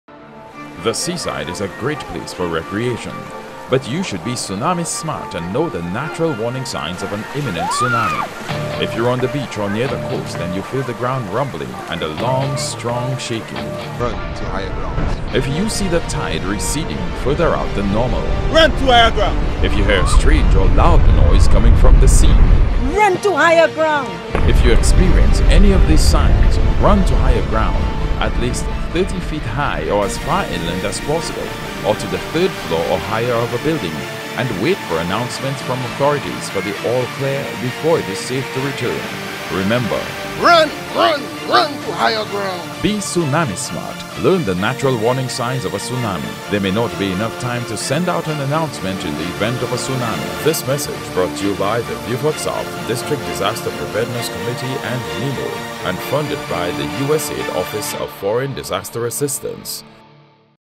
Tsunami warning signs (English version)
A public service announcement by the National Emergency Management Organization. "Be Tsunami Smart"